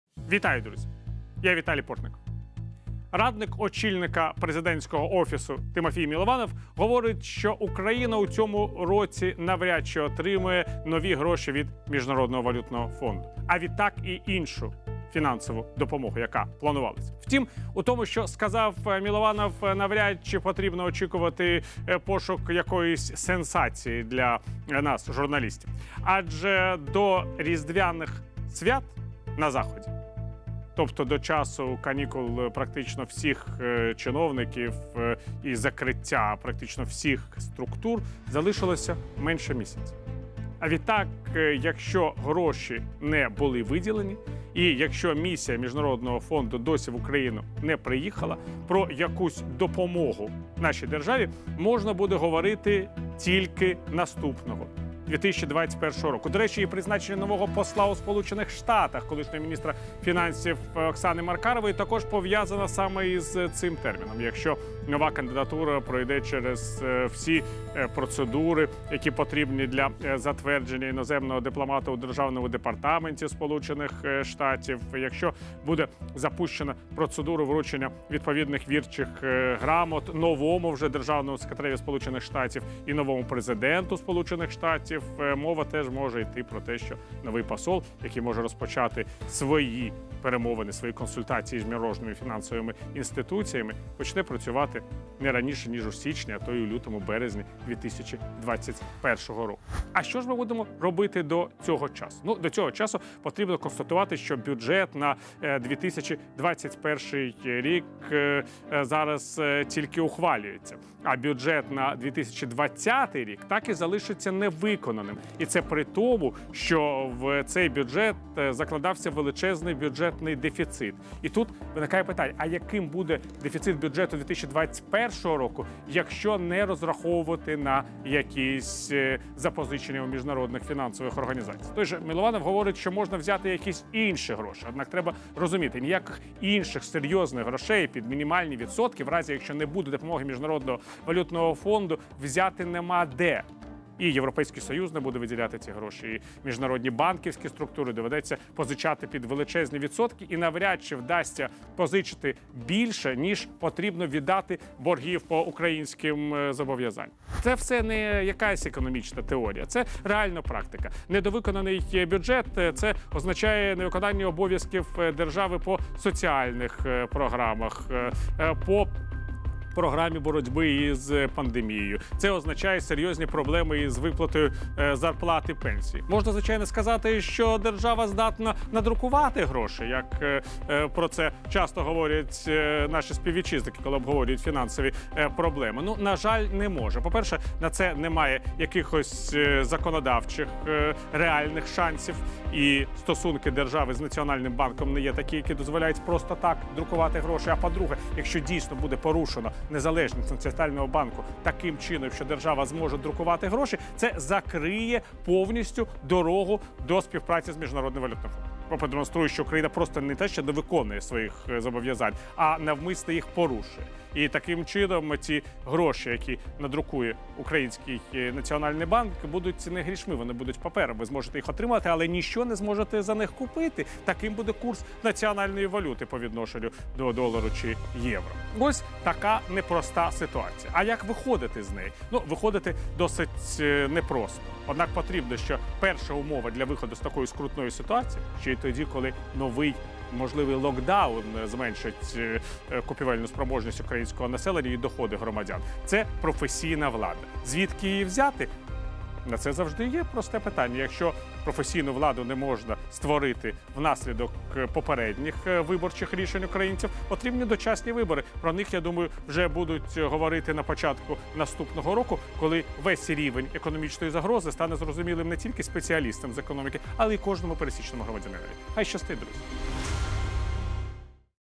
Траншу МВФ не буде. Що робити Україні? | Віталій Портников
Україна не встигає отримати транш від Міжнародного валютного фонду у 2020 році. Бюджет цього року залишиться невиконаним. Також не відомо, яким буде дифіцит бюджету 2021 року. І все це на тлі пандемії коронавірусу. Де уряду брати гроші на соціальні виплати і сплату боргів. Ситуацію аналізує політичний оглядач Радіо Свобода Віталій Портников. Відеоблог «Точка зору».

Думка автора відеоблогу не обов'язково відображає позицію Радіо Свобода.